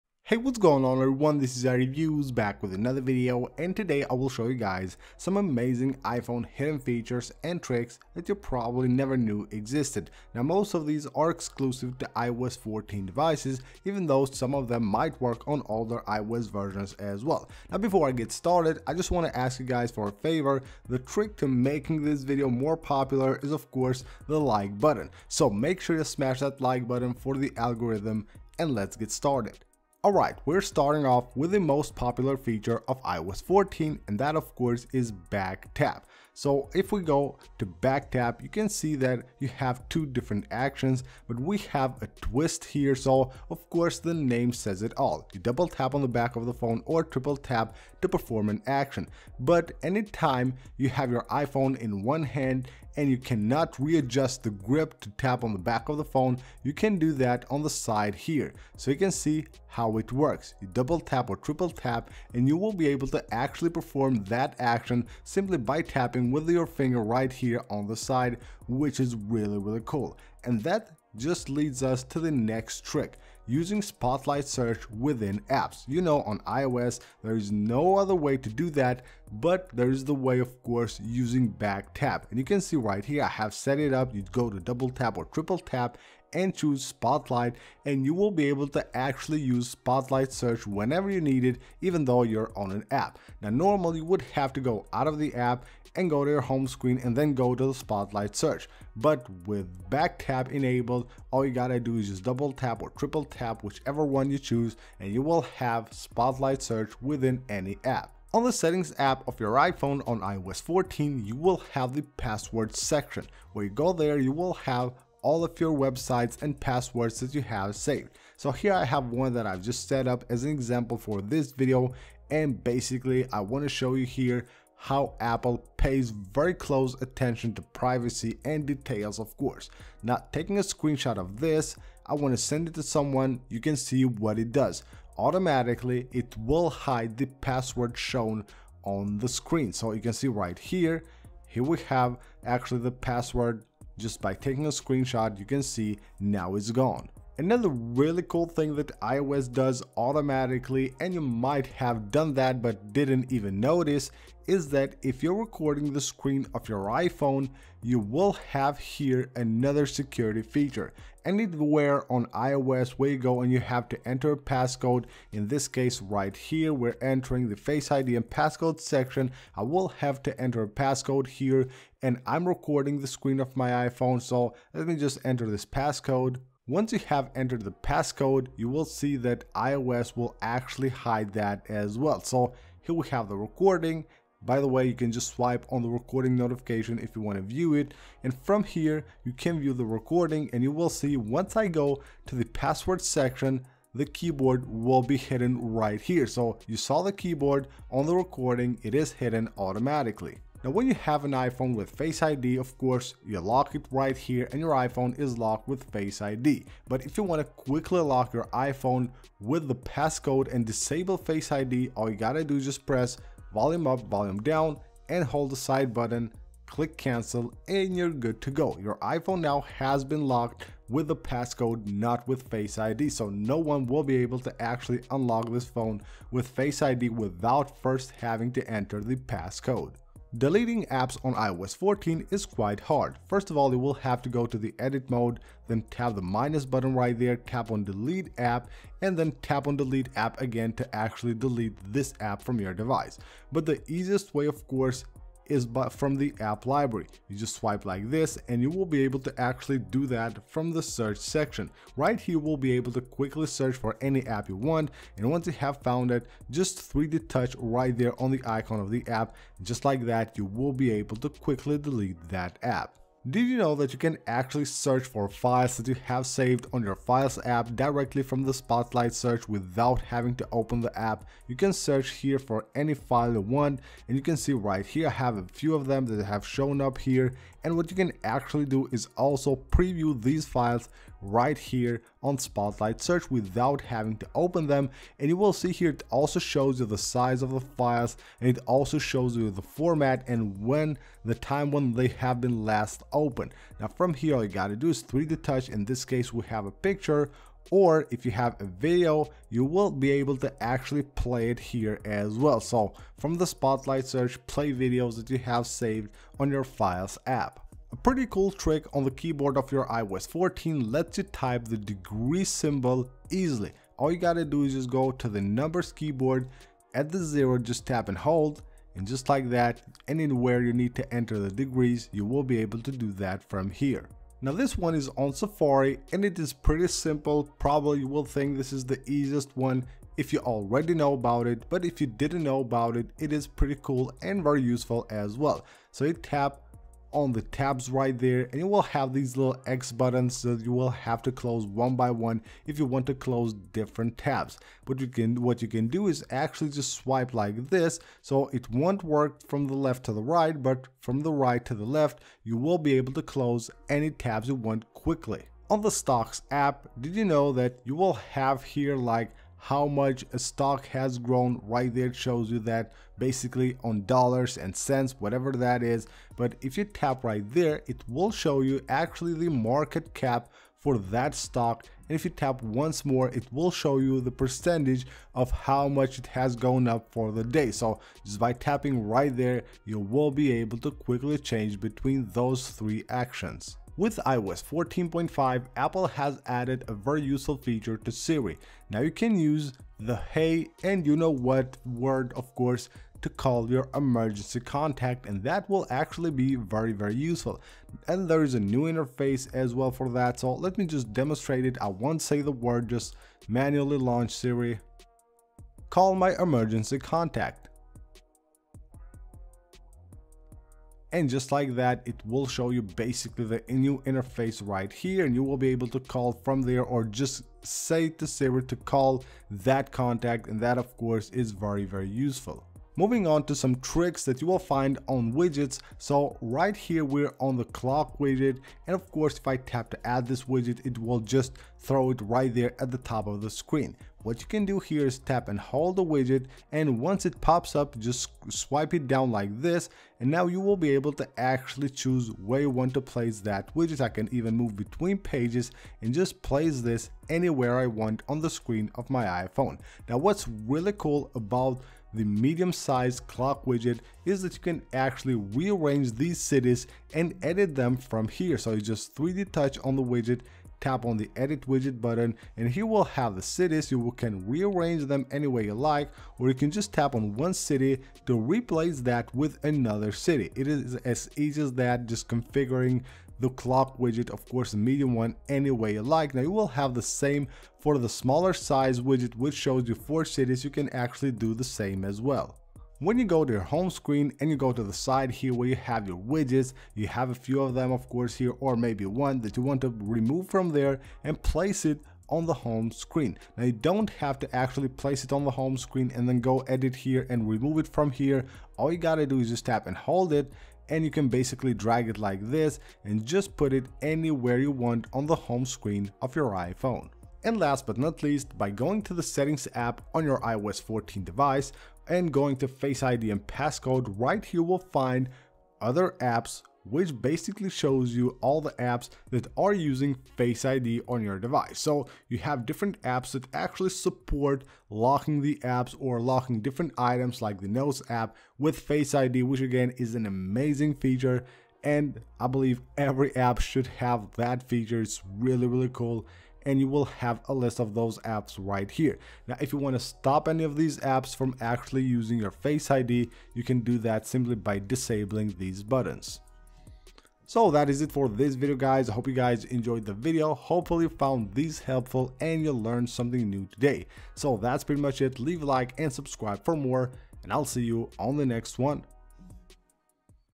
iPhone HIDDEN Features & Tricks | iOS 14 Secrets !
iPhone hidden features and tricks you didn't know existed. iOS 14 has a lot of hidden tricks and secret features that are very useful. In this video you can find 15 iPhone tips and tricks you probably didn't know existed on iOS 14.

Get more out of your iOS 14 device by utilizing these hidden iOS 14 tips and tricks. Some of these might also work on lower iOS versions.